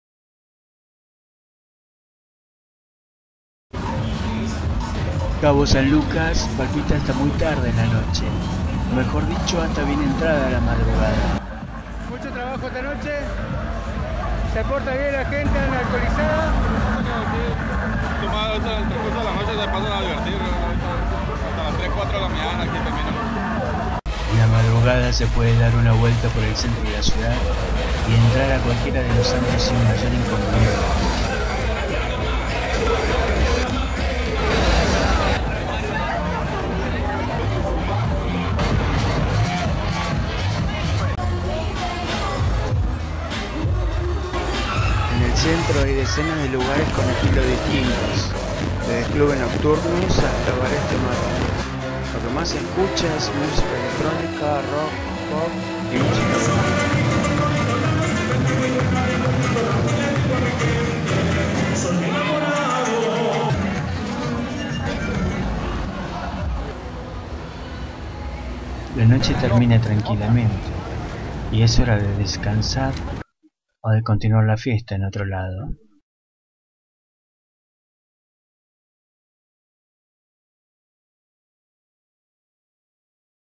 Sábado a la noche en Cabo San Lucas
En Cabo San Lucas, la fiesta del sábado dura hasta más de las 4 de la mañana del otro día. Éste video fue grabado entre las 2 y 4 am del domingo 16/06/2013. Sirve de referencia para los que quieren conocer Los Cabos.